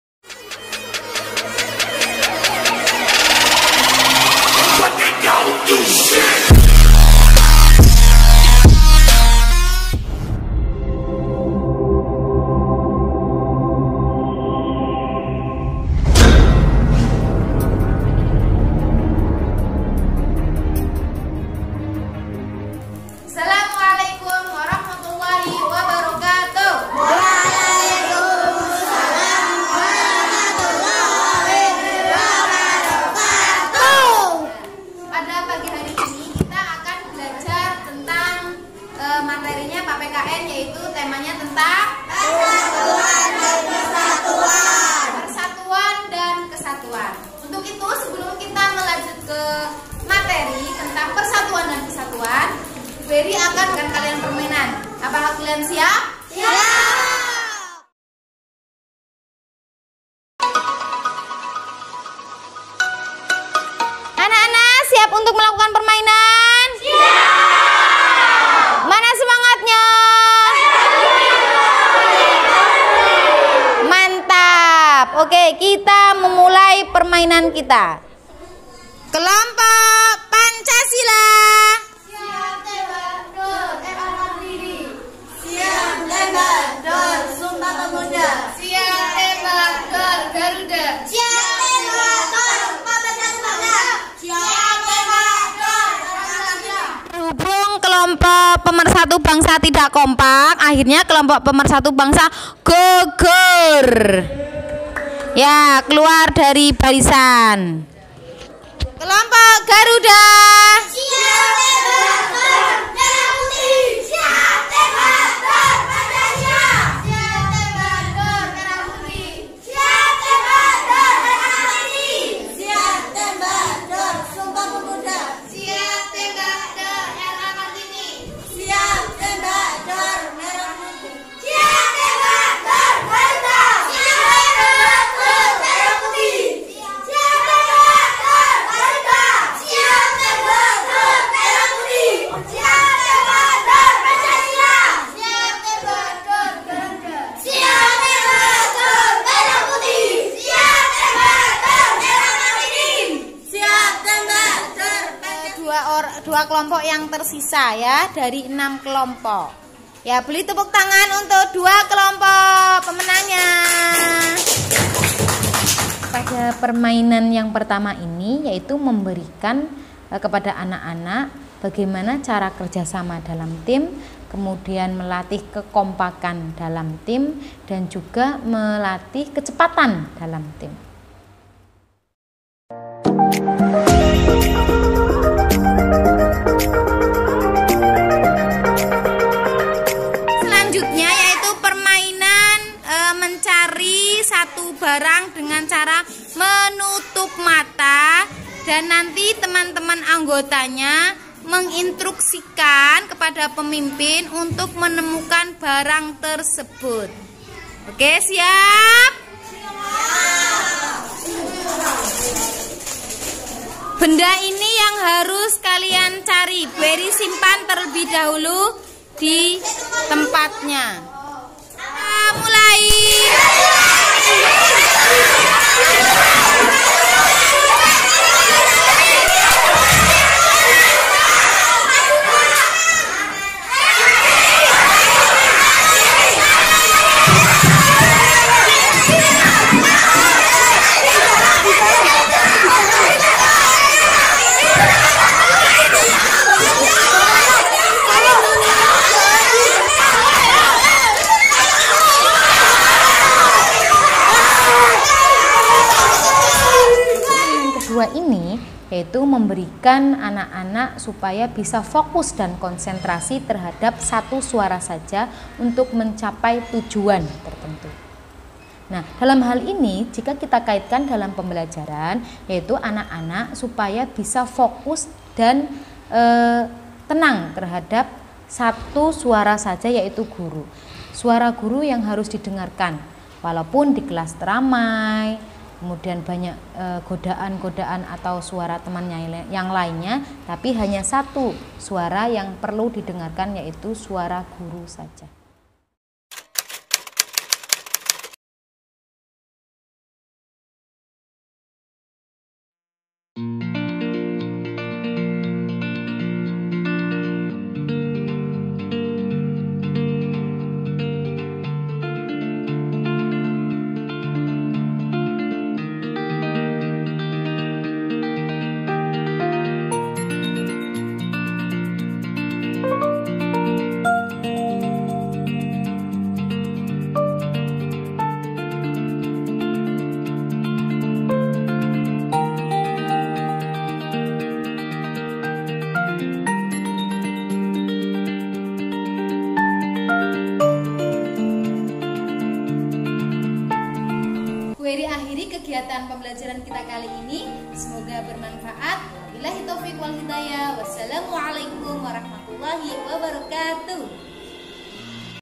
Kreasi Pembelajaran Seru Permainan Persatuan dan Kesatuan Kelas 5A Ali Bin Abi Thalib
Belajar sambil main tembak tembakan? dor dor dor. Mau tau keseruannya bermain tembak-tembakan sambil belajar bersama bu ery? cekidot mari kita simak pembelajaran PPKn kreasi membuat mading persatuan dan kesatuan